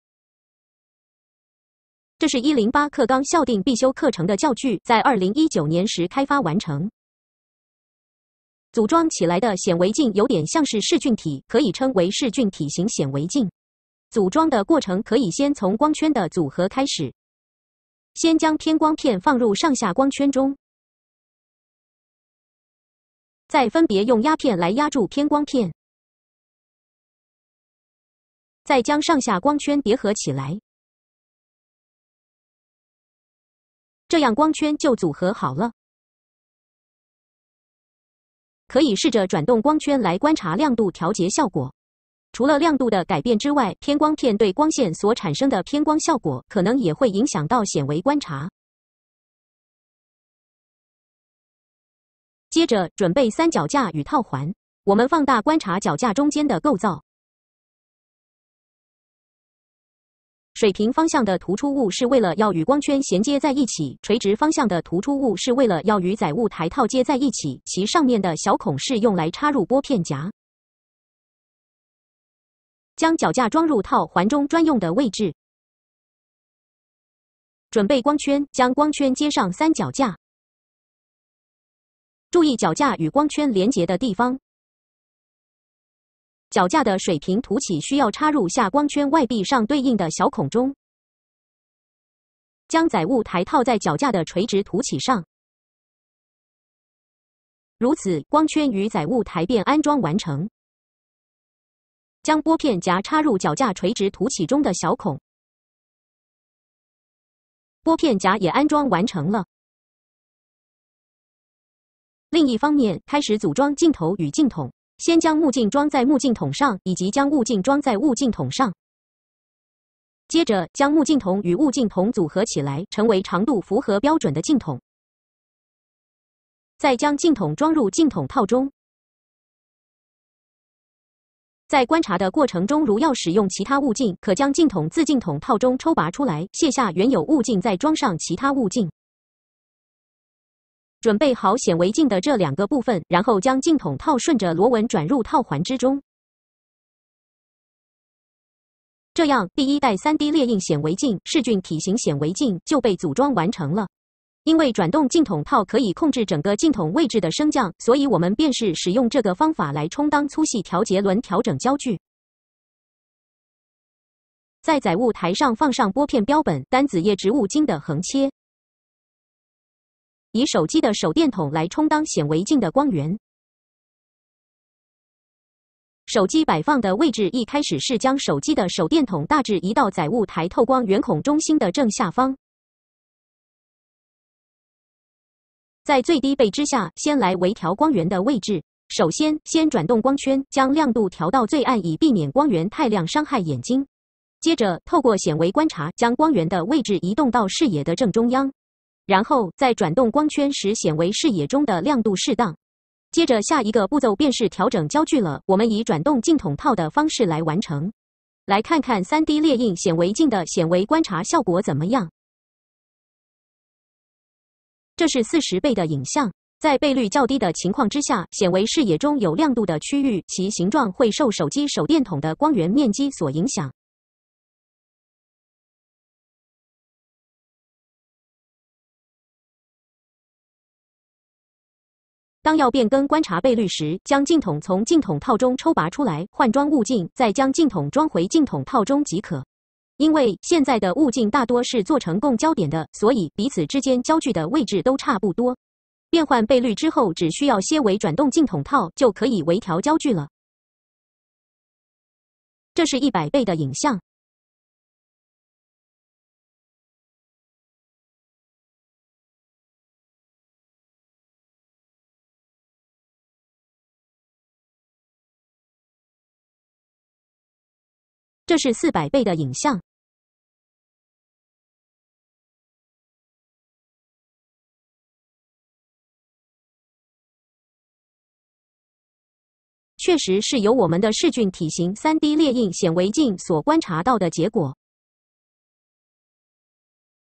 3D列印顯微鏡的組裝與使用_3d printed microscope(重放)
這一台是自發明的3D列印顯微鏡，稱為噬菌體型顯微鏡。

由於它的形狀與一般的顯微鏡頗為不同，作為一個教具，它可以啟發學生對於顯微鏡的光學原理與作動機構產生許多的想法。

然而在使用上，它仍有幾個需要改進的地方：
1.以轉動鏡筒的方式調節焦距，容易造成鏡筒晃動。
2.以手機的手電筒作為光源，可能會造成顯微影像的解析度下降。
3.以兩偏光片的相對角度調節亮度，同時也產生了偏光的效果。
4.當初為了節省材料，骨架的設計相對單薄，整個機體感覺不太堅固。